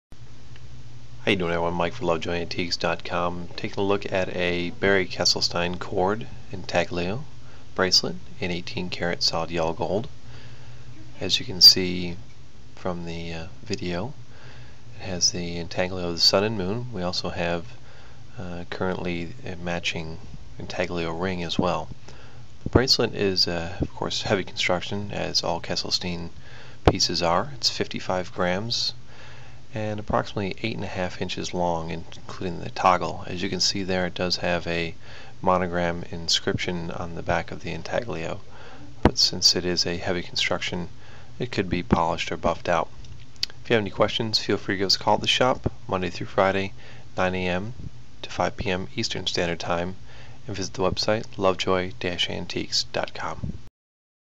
Barry Kieselstein-Cord 18k Intaglio Bracelet Sun & Moon 8.5" 55 Grams
Material: 18k Solid Gold

Size: 8mm wide links. 8.5" long.

Condition: Good

Grams: 55 grams

This listing is for an authentic Barry Kiesselstein Intaglio Bracelet. They are very well made and extremely stylish.